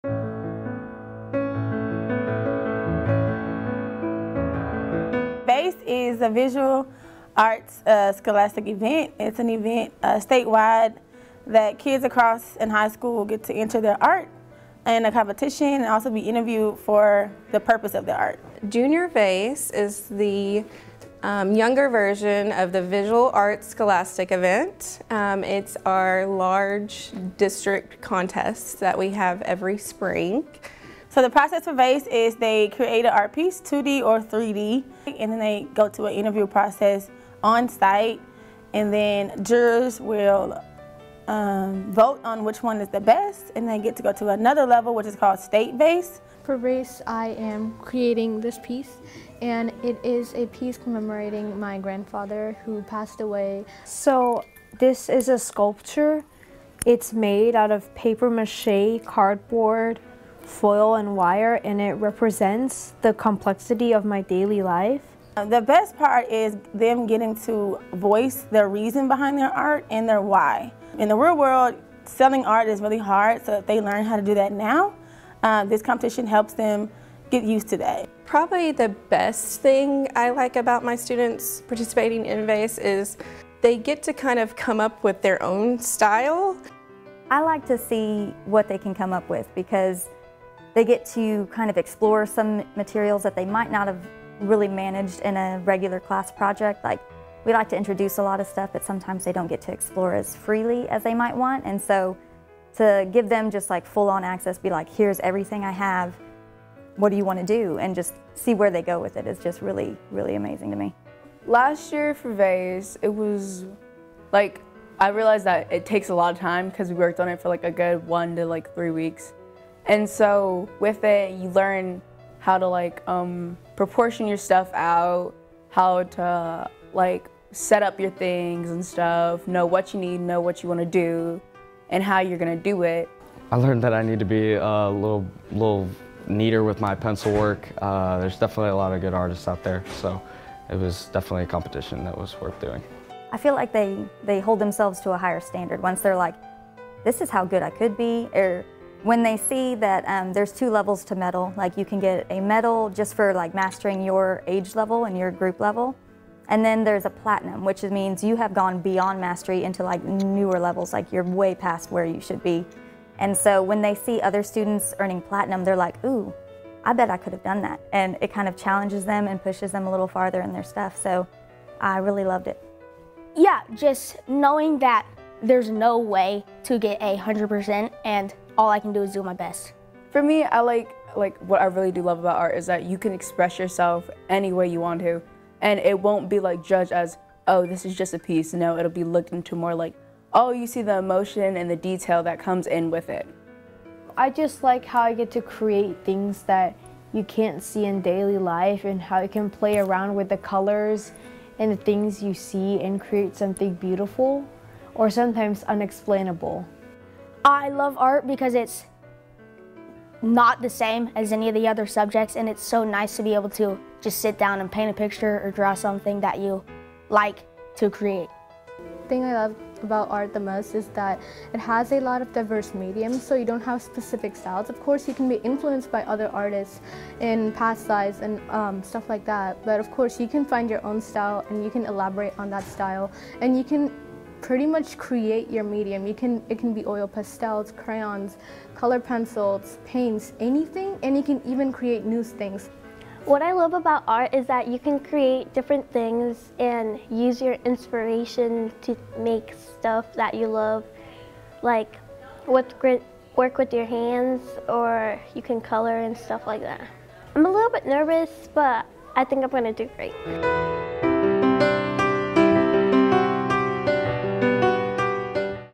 VASE Art - High School & Middle School
Visual Arts Scholastic Event, also known as VASE, is an annual state-wide event that high school art students participate in to showcase their artistic talents and communication skills.

“The best part is them getting to voice their reason behind their art and their why.” stated McKinney North Art teacher, Coach Kelley.

While high school students participate in state VASE, middle school students have the opportunity to participate in Junior VASE, which is a district-wide version of the competition.

McKinney North VASE participant, Keyla Rangel, will advance to the state competition to be held on April 17th.

March 2024